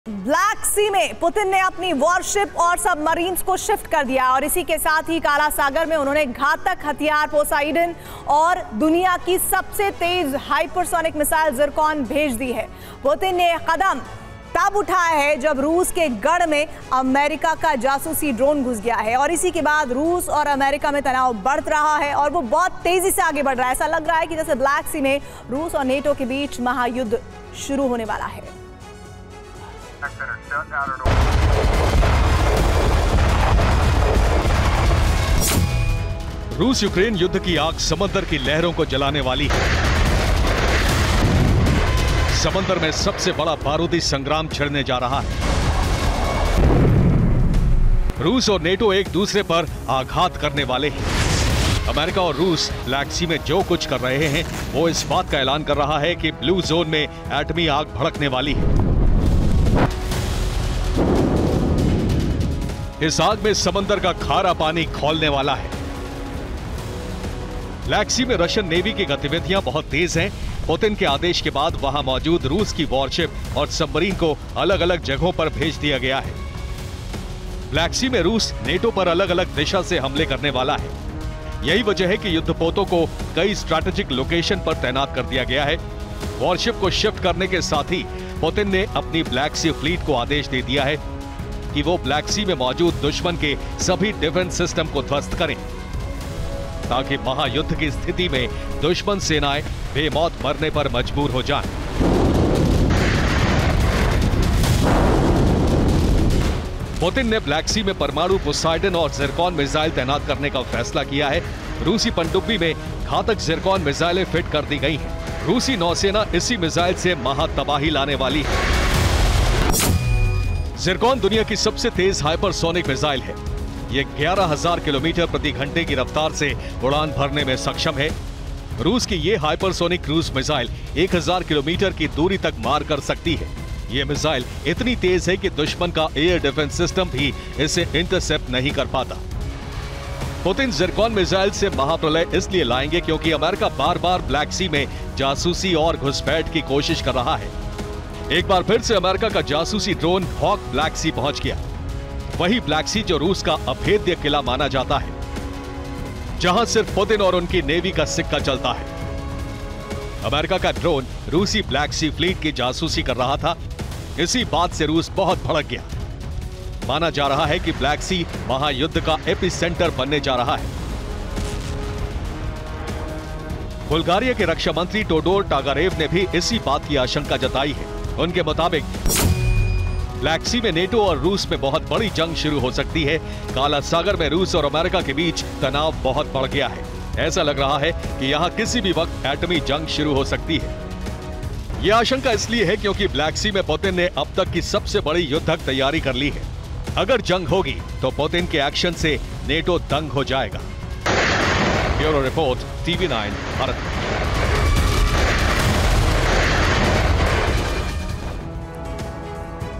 Russia Ukraine War Update: पहुंची Putin की 'ज़िरकॉन', NATO से इंतकाम! | America | Zelensky
ब्लैक सी में पुतिन ने अपनी वॉरशिप और सबमरीन्स को शिफ्ट कर दिया है। इसी के साथ ही काला सागर में उन्होंने घातक हथियार पोसाइडन और दुनिया की सबसे तेज हाइपरसोनिक मिसाइल जिरकॉन भेज दी है। पुतिन ने ये कदम तब उठाया, जब रूस के गढ़ में अमेरिका का जासूसी ड्रोन घुस गया। इसी के बाद रूस और अमेरिका में तनाव बहुत बढ़ गया है। ऐसा लग रहा है जैसे ब्लैक सी में रूस और नाटो के बीच महायुद्ध शुरू होने वाला है।